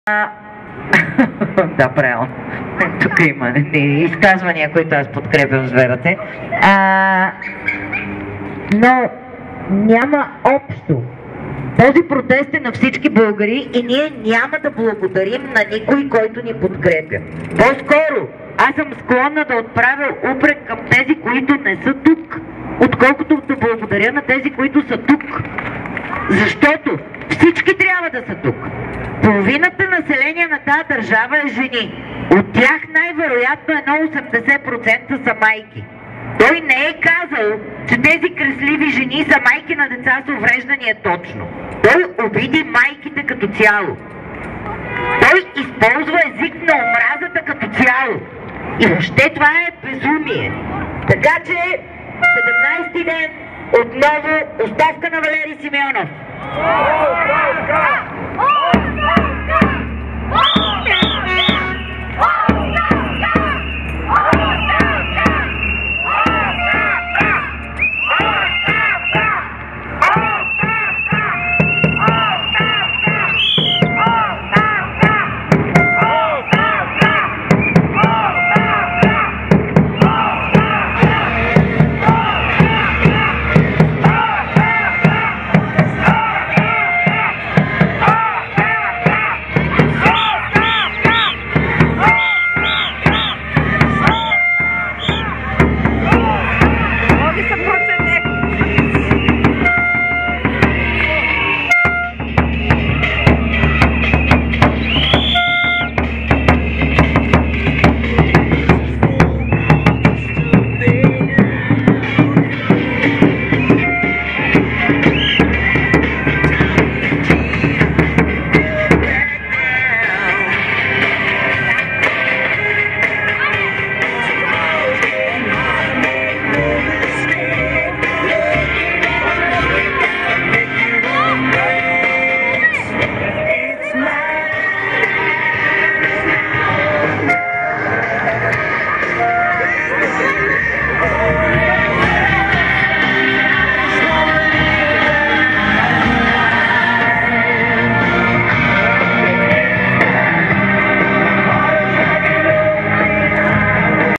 Искане за подкрепа на протеста организиран от Системата ни убива
Оставка на вицепремиера Валери Симеонов!